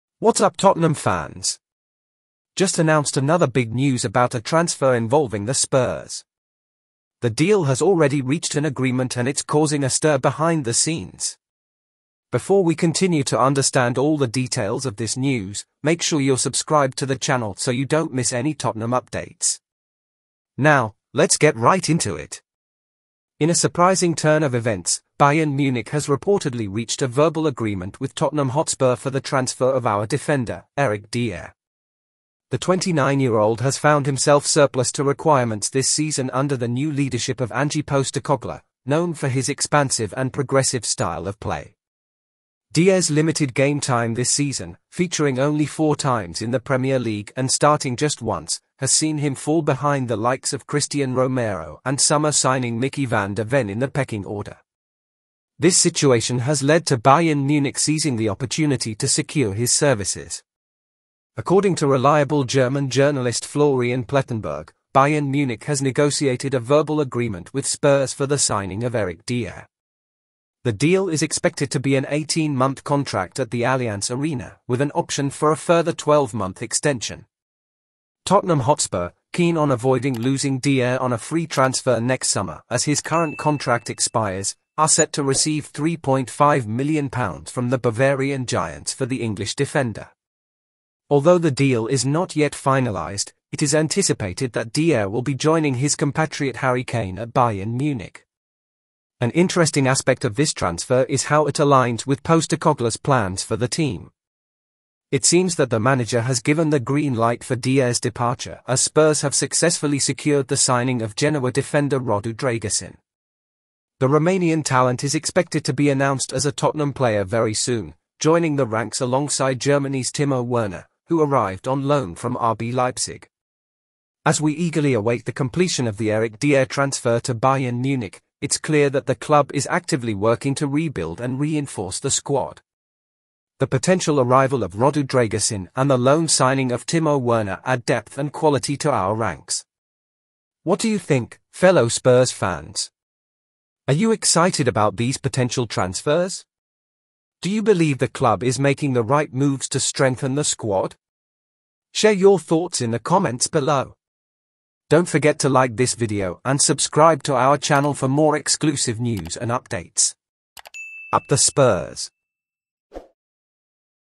💣🚨ANNOUNCED NOW! DEAL CLOSED! PACKED AND READY FOR TRANSFER! TOTTENHAM TRANSFER NEWS! SPURS NEWS!
⚽ What's up Tottenham fans! Just announced another big news about a transfer involving the Spurs! The deal has already reached an agreement and it's causing a stir behind the scenes!

📰 News Credits: sportsmole

Reuters